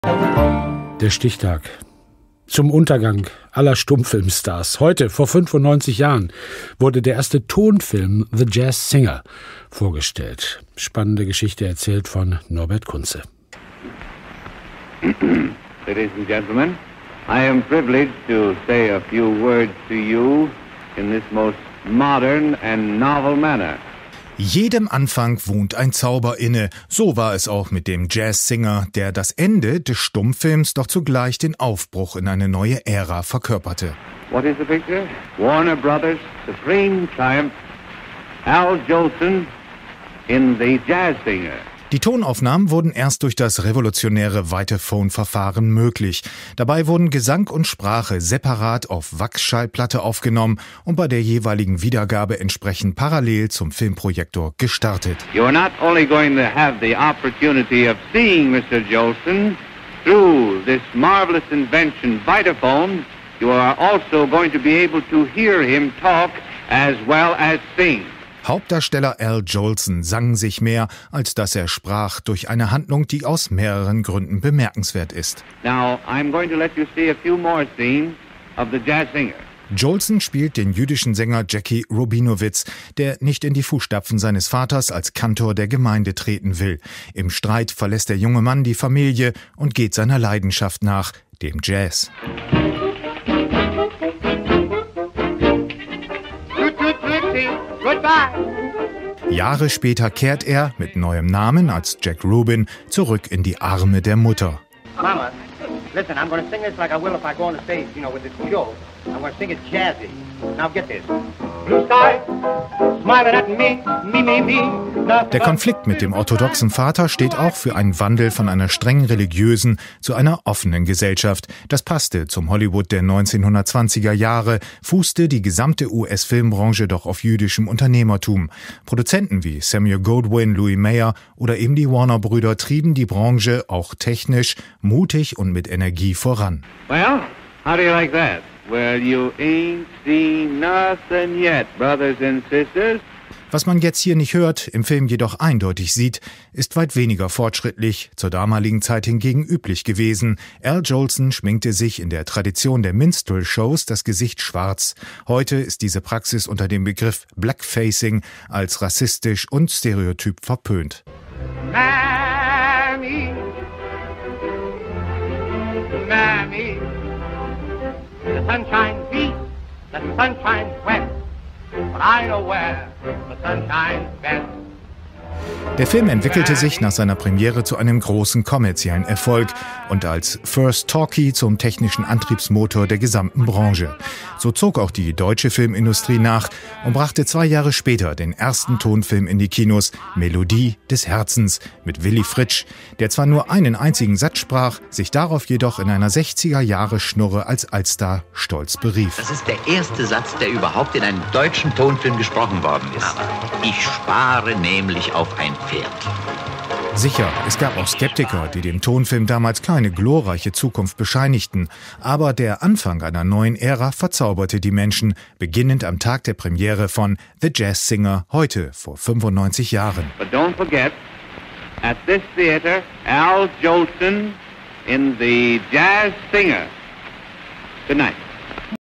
6.10.1927: Der Erste Tonfilm "The Jazz Singer"
Eigentlich als Stummfilm geplant, sollte "The Jazz Singer" nur einige Gesangseinlagen enthalten. Doch Hauptdarsteller Al Jolson redete einfach los. Die Worte blieben drin – und der erste Tonfilm war da. Von Norbert Kuntze.